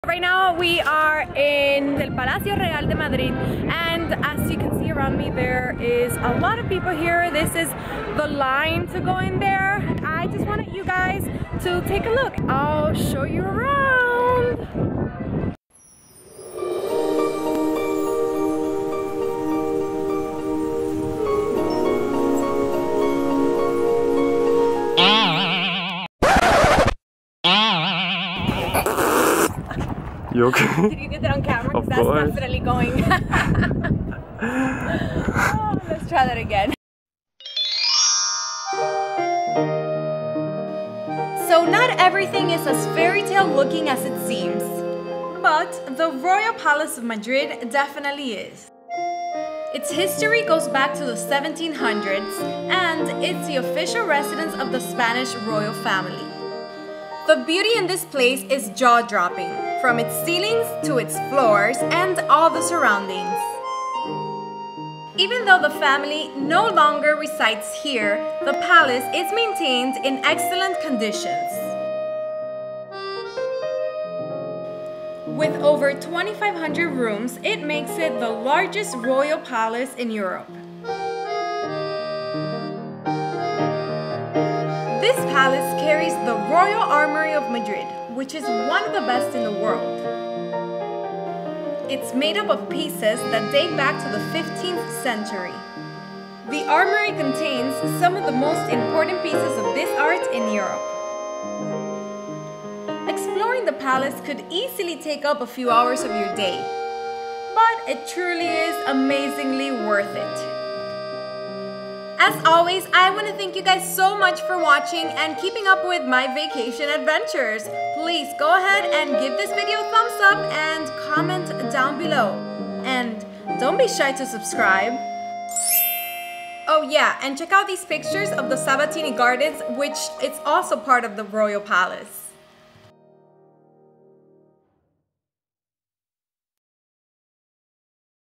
Vacation Adventures: Royal Palace of Madrid! ❤
Sit-back, Relax & Revel in the beauty of royalty and everything the Royal Palace of Madrid has to offer. Go behind the scenes on this exciting & fun vacation adventure! The Royal Palace of Madrid, or as locals call it, el “Palacio Real de Madrid”, was what impressed me the most on my trip!

Make sure to view this short video; it’ll come in handy when planning your next visit to Madrid, Spain.

Don't forget to leave your feedback below.

XOXO,

Jenny Arzola

Periscope @Jenny Arzola